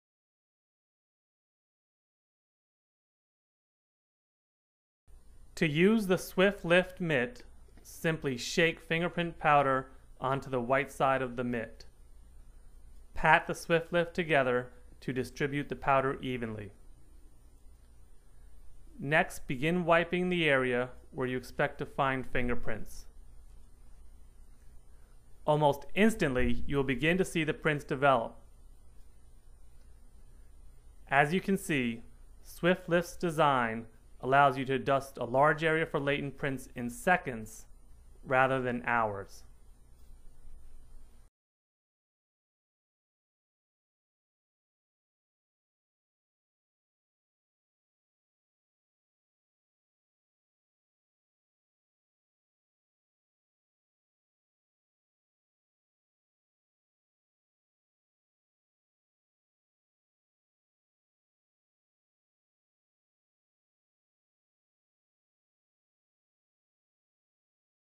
Swift-Lift Latent Print Developing Mitt
Demonstration of Tri-Tech Forensics' Swift-Lift Latent Print Developing Mitt